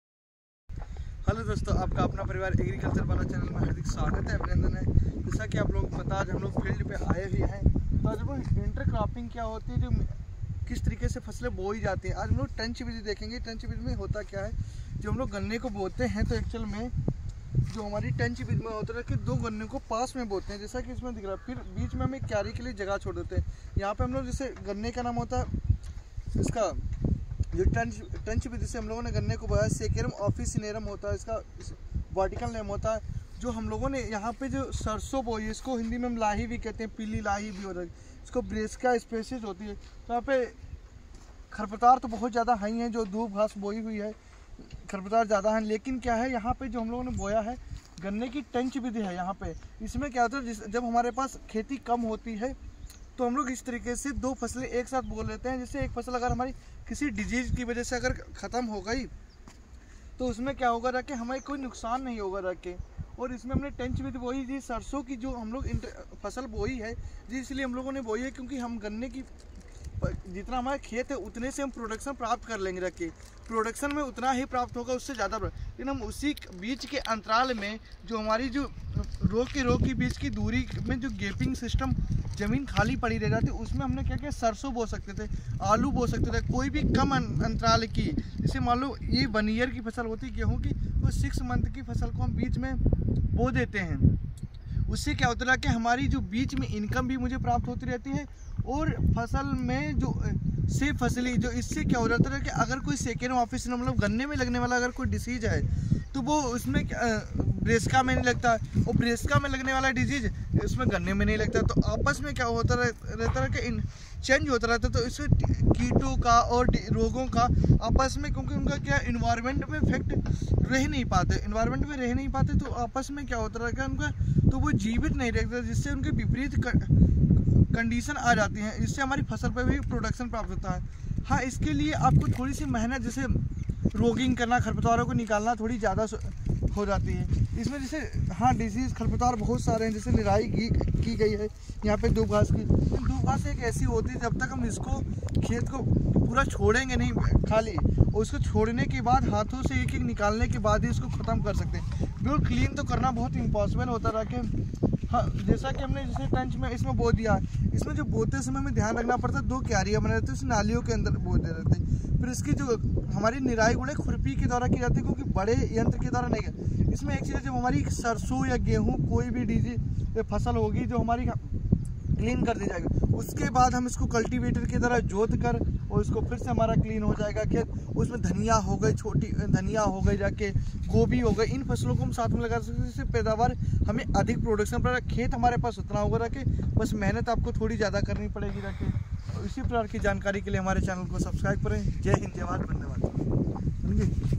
Intercroping method ll गाना और सरसों की खेती एक ही खेत में टंच विधि से गाना बोलने की बुवाई ll#agri
एक ही खेत में गाने और सरसों की खेती इंटरक्रॉपिंग मेथड से किस प्रकार की जाती है
इंटरक्रॉपिंग मेथड क्या होता है